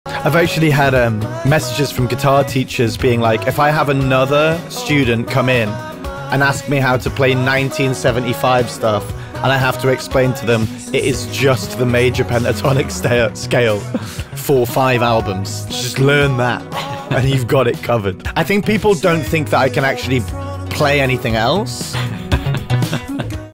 Can Matty Healy Really Play Guitar?
Can Matty Healy play anything other than the major pentatonic scale? Guitar teaches don't think so!